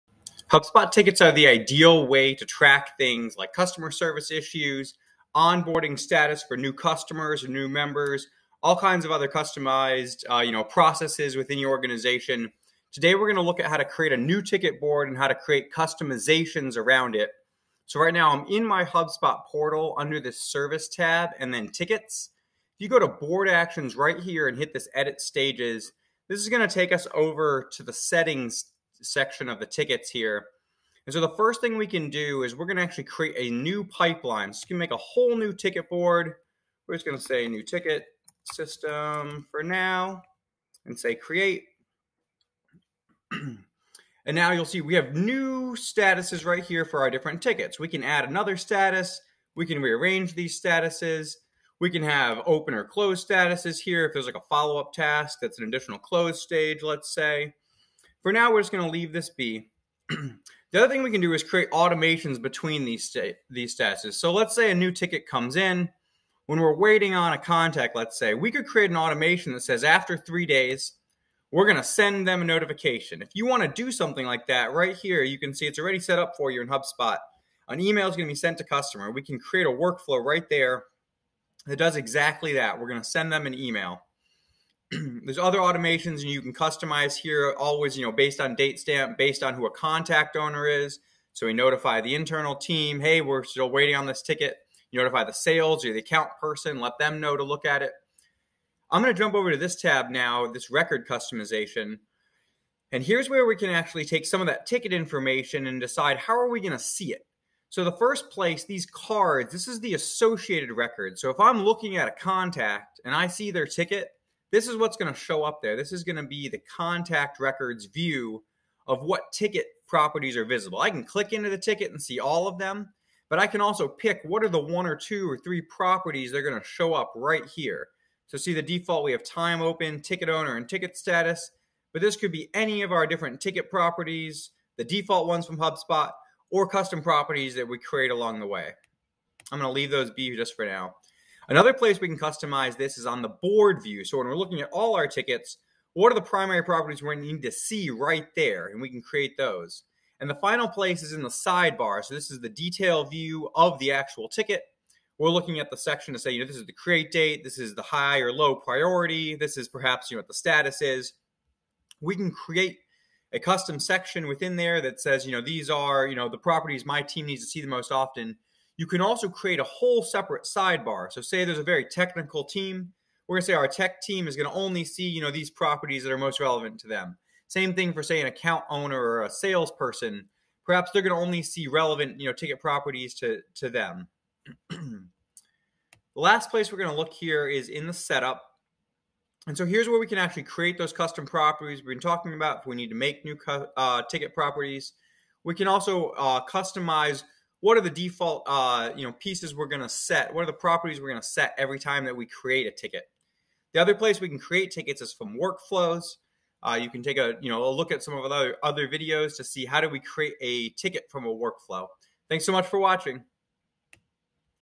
HubSpot Basics - Creating a Ticket Board and Customizing Your Views in HubSpot CRM
Create a new ticket board in HubSpot CRM, customize the status and properties, and trigger automations from workflows.

0:32 Create a new ticket pipeline
1:08 Include automation based on ticket status
1:40 Customize ticket records and views
2:41 Add section to your sidebars
3:08 Custom ticket properties